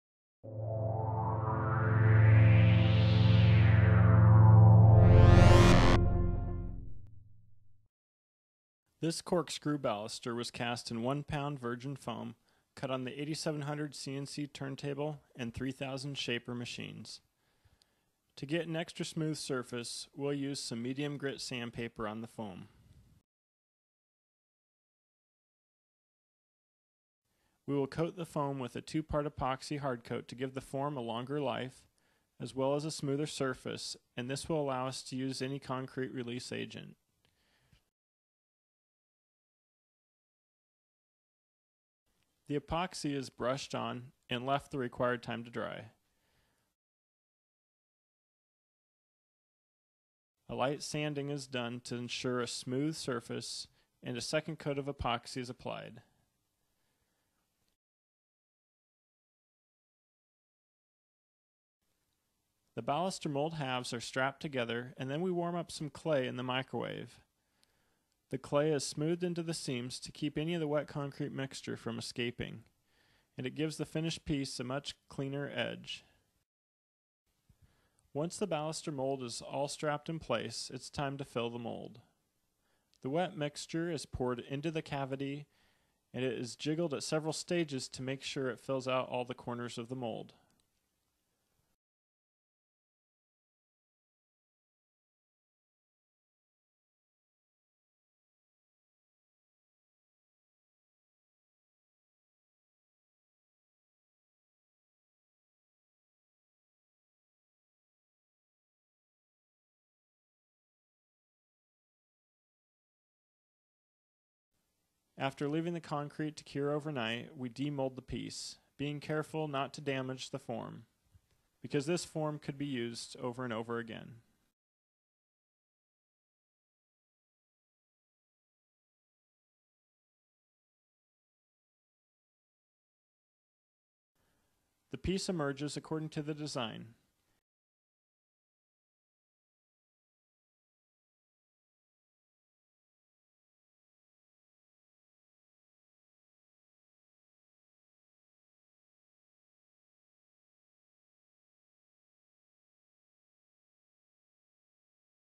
Precast Concrete Baluster EPS Foam Mold
Using a the hot wire 8700 CNC and hot wire 3000 Shaper and expanded polystyrene (EPS) foam, a custom architectural precast concrete baluster mold is created. This video shows it being prepared with an epoxy hard coat and poured. This technique applies to decorative and ornamental architectural concrete, cultured stone, and GFRC applications.